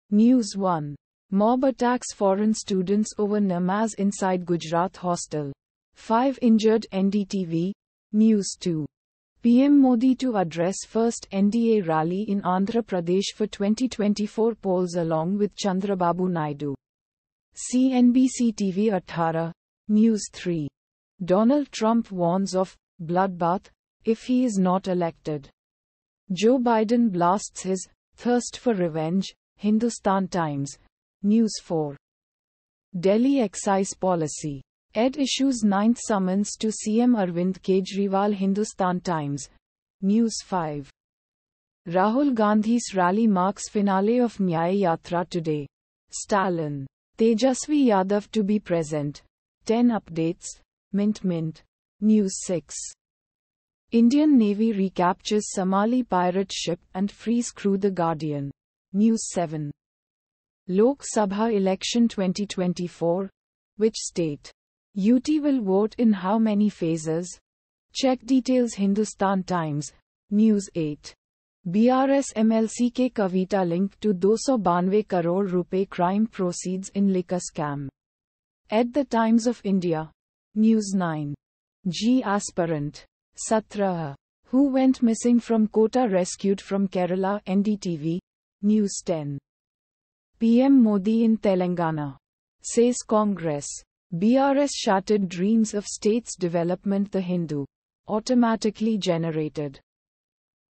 News Headlines (India)- 18/03/2024,11:49 AM GMT+5:30
News 1:Mob Attacks Foreign Students Over Namaz Inside Gujarat Hostel, 5 Injured - NDTV
News 2:PM Modi to address first NDA rally in Andhra Pradesh for 2024 polls along with Chandrababu Naidu - CNBCTV18
News 3:Donald Trump warns of ‘bloodbath’ if he is not elected, Joe Biden blasts his ‘thirst for revenge’ - Hindustan Times
News 4:Delhi excise policy: ED issues 9th summons to CM Arvind Kejriwal - Hindustan Times
News 5:Rahul Gandhi's rally marks finale of Nyay Yatra today: Stalin, Tejashwi Yadav to be present | 10 updates | Mint - Mint
News 6:Indian navy recaptures Somali pirate ship and frees crew - The Guardian
News 7:Lok Sabha election 2024: Which state, UT will vote in how many phases? Check details - Hindustan Times
News 8:BRS MLC K Kavitha link to Rs 292 crore crime proceeds in liquor scam: ED - The Times of India
News 9:JEE Aspirant, 17, Who Went Missing From Kota Rescued From Kerala - NDTV
News 10:PM Modi in Telangana, says Congress, BRS shattered dreams of State’s development - The Hindu

Automatically Generated...